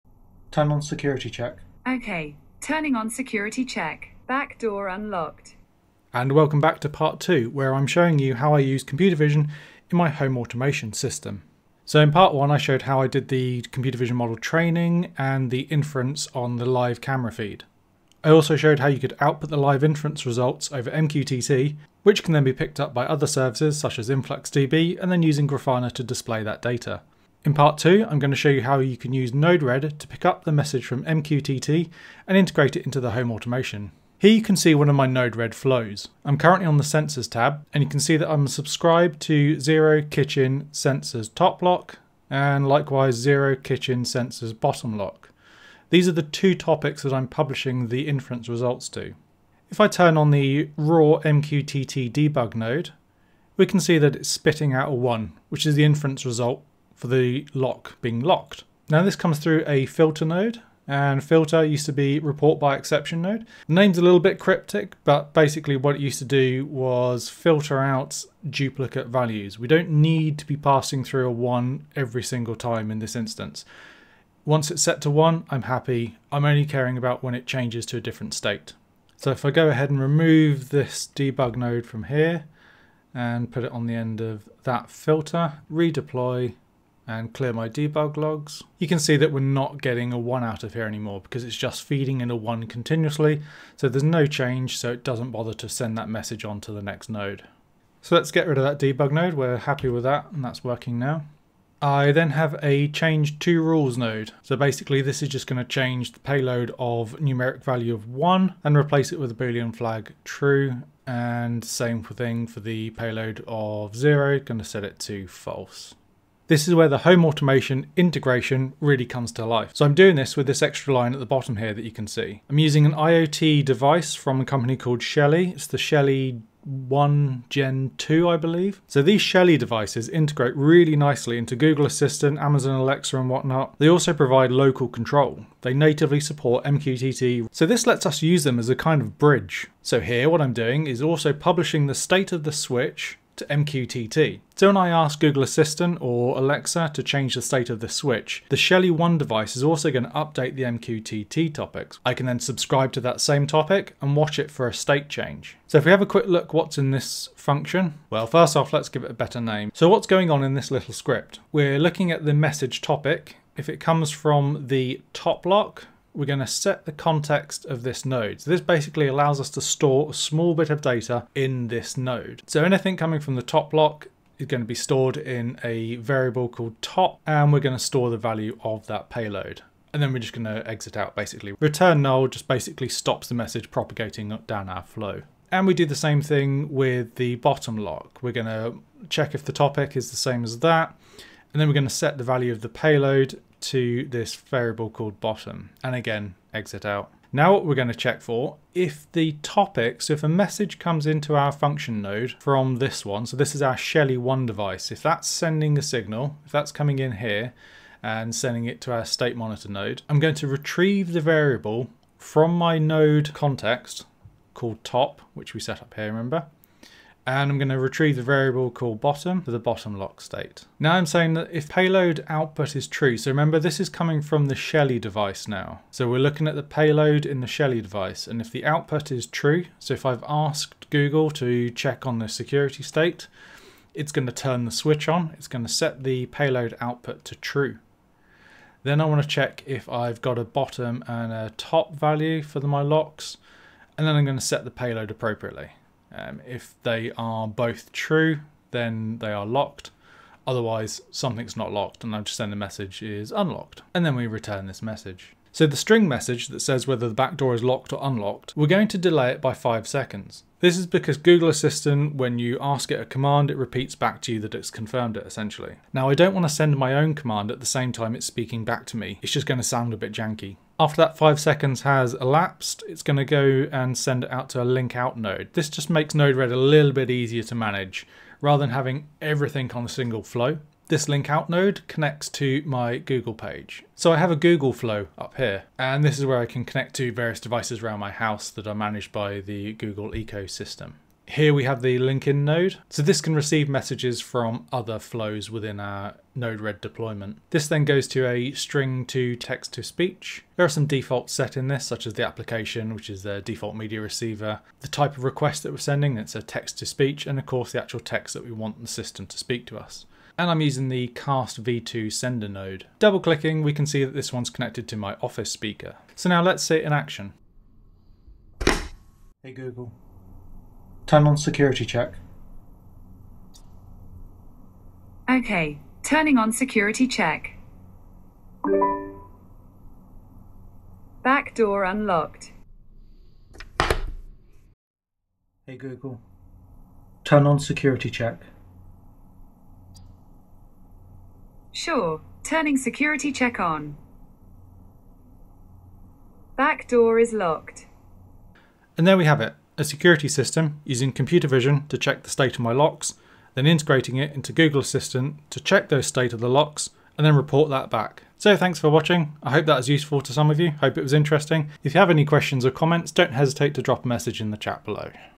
Adding dumb devices to your smart home - Part 2
Using vision as a sensor to monitor the state of legacy, non-connected devices and integrating them into my smart home.

I've been dabbling in home automation for a few years, using the fairly common MING stack (MQTT, influxdB, node-red, Grafana), I've also built a few custom ESP based sensors.

I'm now exploring using computer vision as a sensor to monitor things that aren't connected or 'smart' enabled yet.

I've trained an object detection model to watch my CCTV IP cameras, it finds the locks on my back door and uses a second model to classify the state. I then do the usual publish the results over mqtt... the rest is history after that, getting fed into the MING stack.

Chapters:
00:00 Intro
00:11 Recap
00:35 Node-RED
02:02 Shelly IoT bridge
02:35 Node-RED Script
04:40 Google Integration by proxy
05:16 The Result